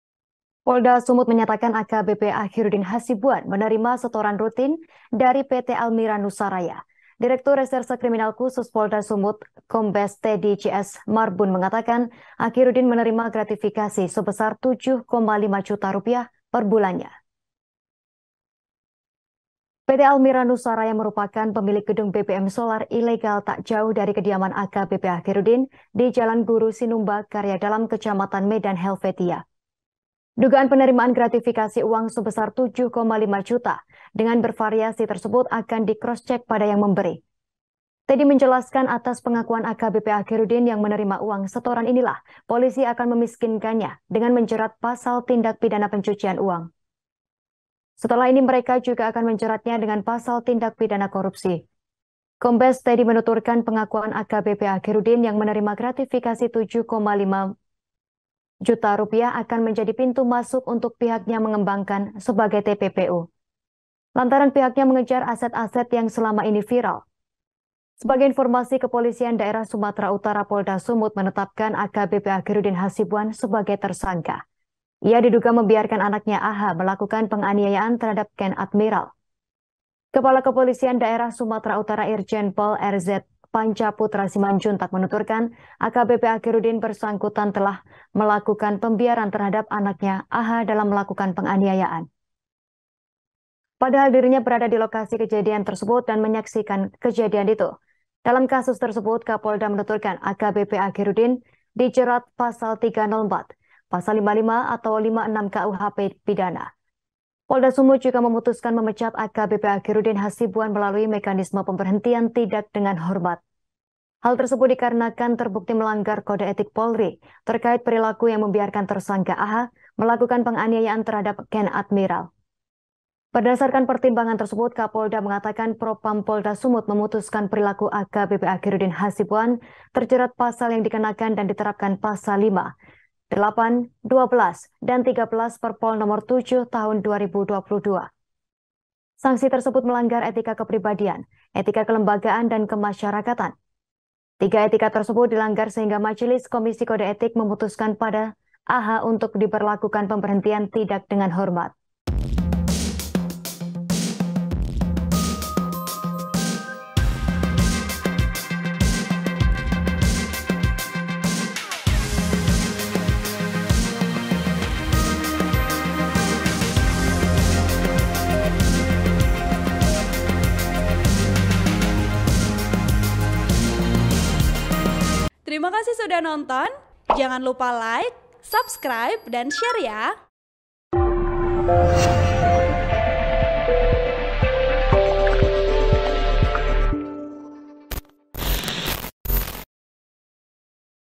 Dari Bisnis Gudang Solar Ilegal Medan, AKBP Achiruddin Ngaku Terima Setoran Rp 7,5 Juta Perbulan
TRIBUN-VIDEO.COM- Polda Sumut menyatakan, AKBP Achiruddin Hasibuan menerima setoran rutin dari PT Almira Nusa Raya, selaku pemilik gudang BBM solar ilegal tak jauh dari kediamannya di Jalan Guru Sinumba/Karya Dalam, Kecamatan Medan Helvetia.

Direktur Reserse Kriminal Khusus Polda Sumut Kombes, Teddy JS Marbun mengatakan, Achiruddin menerima gratifikasi sebesar Rp 7,5 juta perbulannya.

"Mudah-mudahan dengan dugaan di awal bahwa saudara AH ada menerima gratifikasi uang sebesar 7,5 juta dengan bervariasi ini kita akan kroscek dengan yang memberi,"kata Kombes Teddy JS Marbun, Selasa (2/5/2023) malam.

Teddy menjelaskan, atas pengakuan AKBP Achiruddin yang menerima uang setoran inilah Polisi akan memiskinkannya dengan menjerat Pasal tindak pidana pencucian uang.

Setelah ini mereka juga akan menjeratnya dengan pasal tindak pidana korupsi.